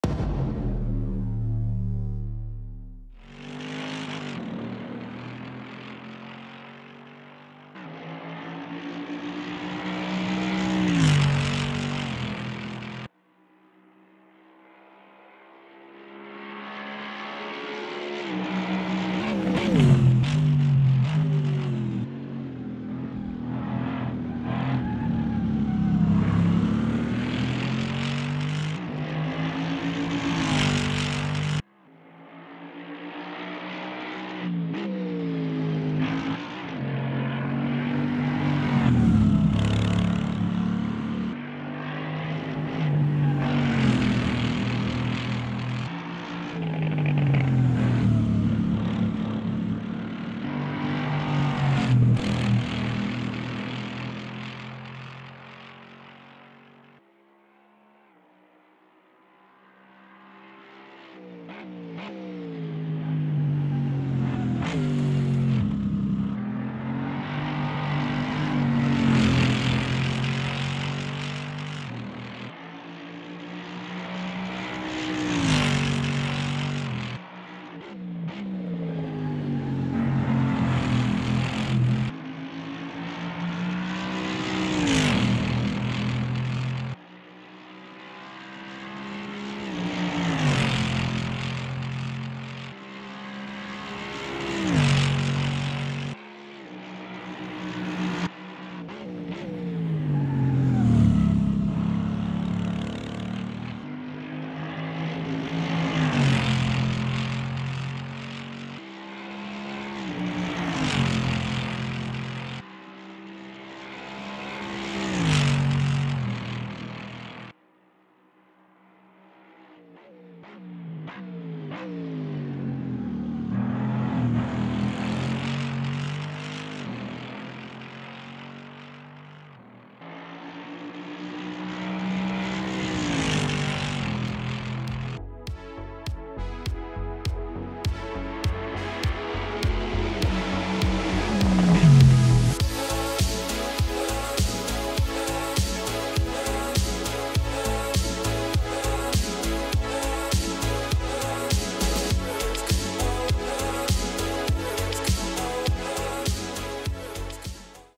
F1 Mercedes Safety Car at Jeddah
In this video I take the Mercedes Safety Car around the Jeddah Corniche Circuit.